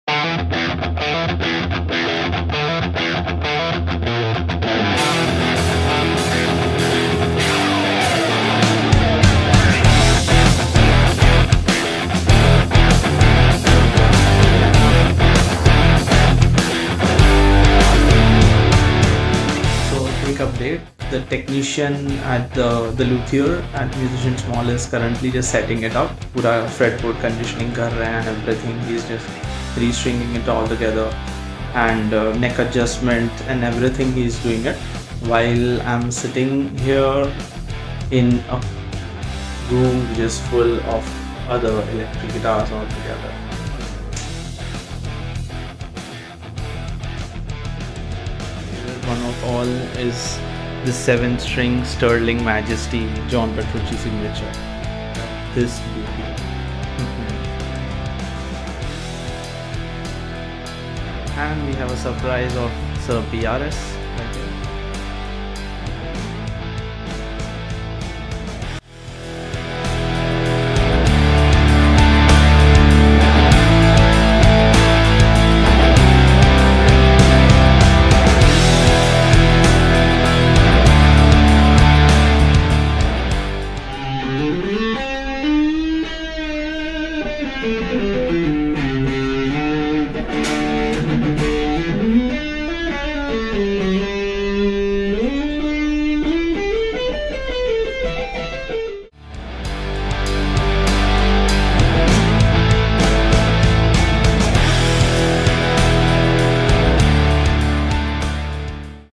I FINALLY FOUND THE ONE | VLOG | 2ND ELECTRIC GUITAR
So I went to purchase my second electric guitar from Musician's Mall, Charni Road.
The owner Ronak Bhargava had been of great help in getting me this model.
It is an Ibanez RGA42EX Black Aurora Burst Matte.

The guitar plays like a beauty, and it is really versatile.
I will also upload a demo of the guitar, explaining all the specs soon.

Keep Supporting :D